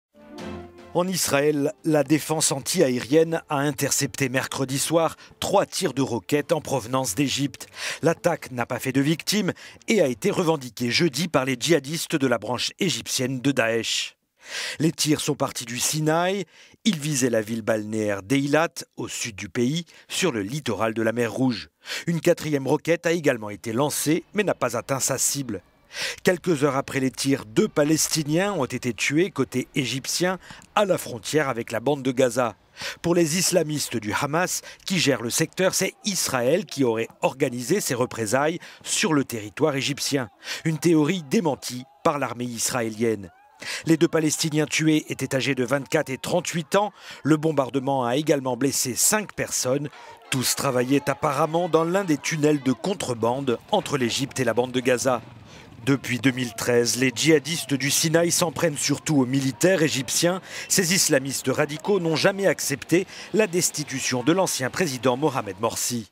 La branche égyptienne de Daesh revendique les tirs sur Israël
Les jihadistes de la branche égyptienne de Daesh ont revendiqué jeudi les quatre tirs de roquettes sur la ville israélienne d'Eilat. Les tirs ont eu lieu mercredi soir au sud du pays, sur le littoral de la mer Rouge.

BREAKING: 3 roquettes tirées contre la station balnéaire d'Eilat interceptées avec succès par le Dôme de Fer (armée de l'air israélienne) Julien Bahloul (@julienbahloul) 8 février 2017

L'attaque n'a pas fait de victime : trois des roquettes ont é…